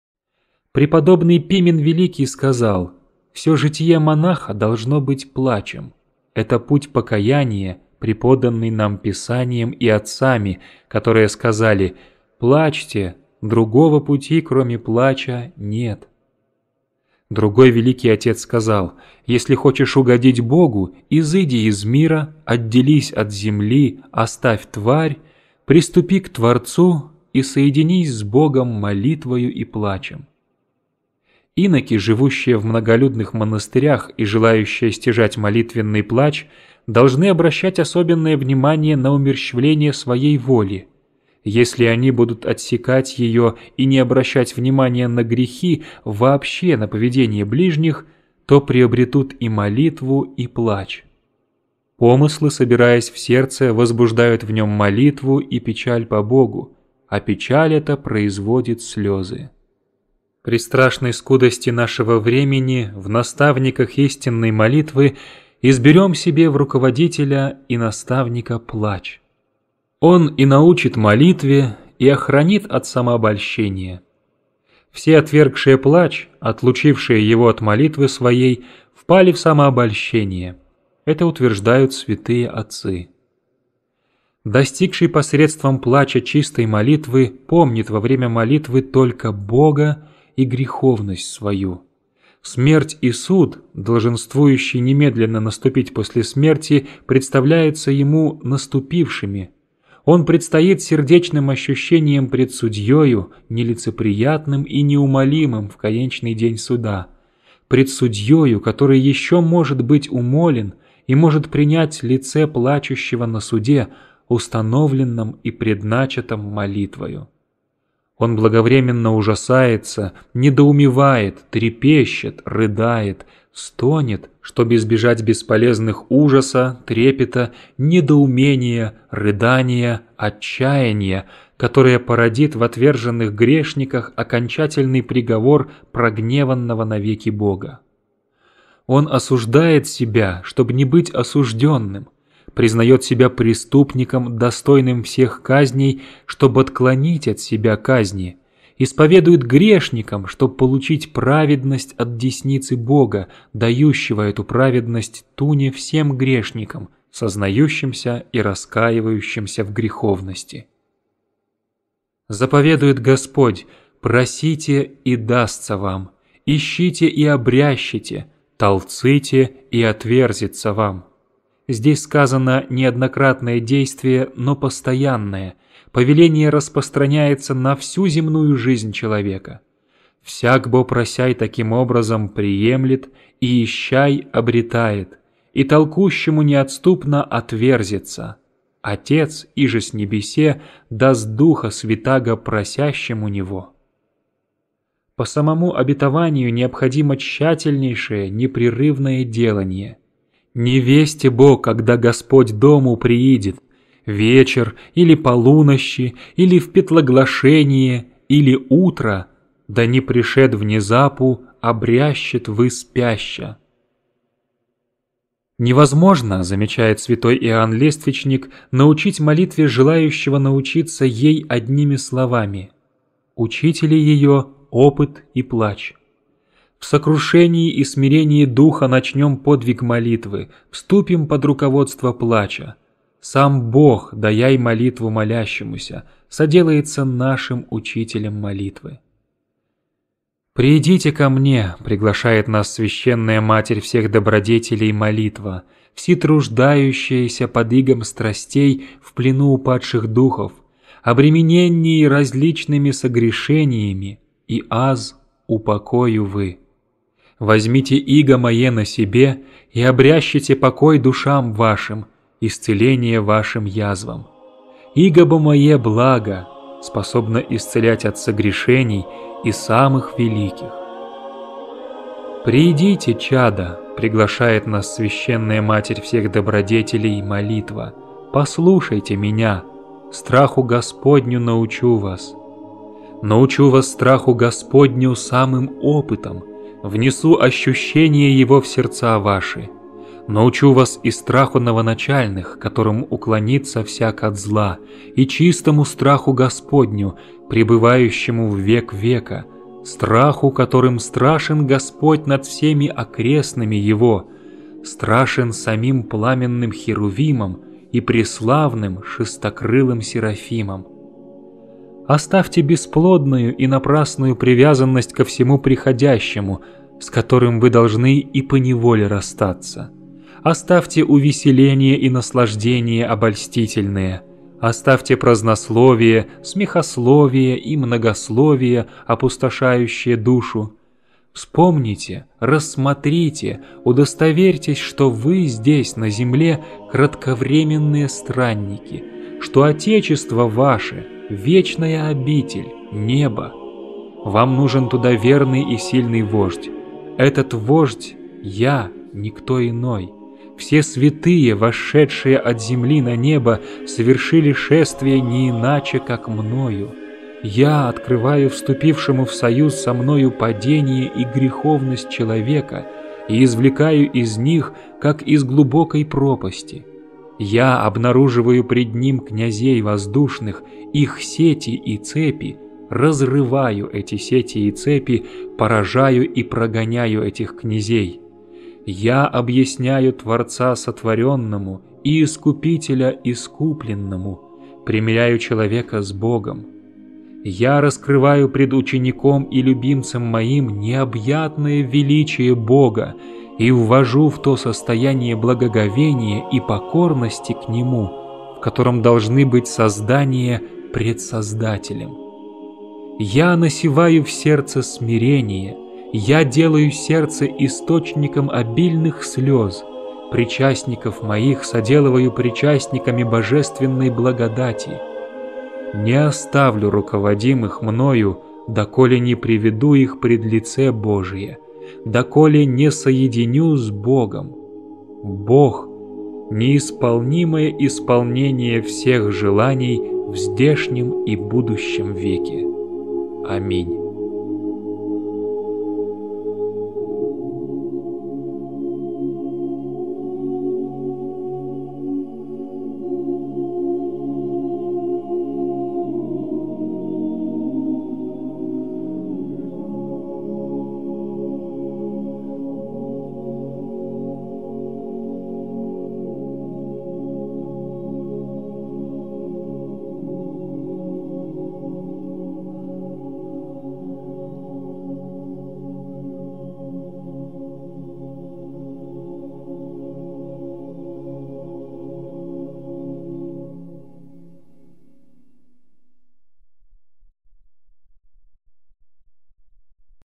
Святитель Игнатий Брянчанинов. О молитве. Часть 13
Игнатий (Брянчанинов) — святитель, который наиболее доступно, для нашего времени, излагает учение о молитве, объясняя ее простоту и необходимость. А также предостерегает от ложных путей самомнения, именуемых прелестью.